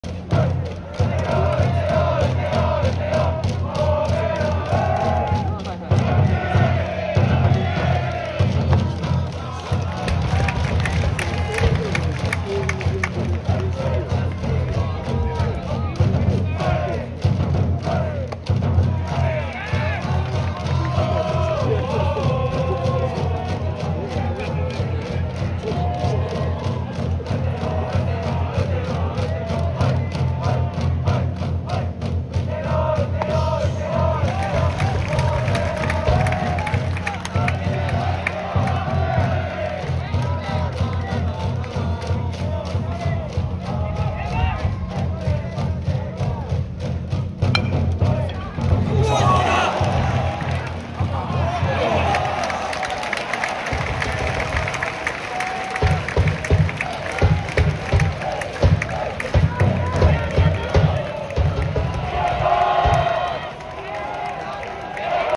慶應義塾、江戸佑太郎ナイスプレー
2024/09/21
慶應義塾対日大藤沢
慶應義塾、江戸、河内の
ファーストゴロをナイスプレー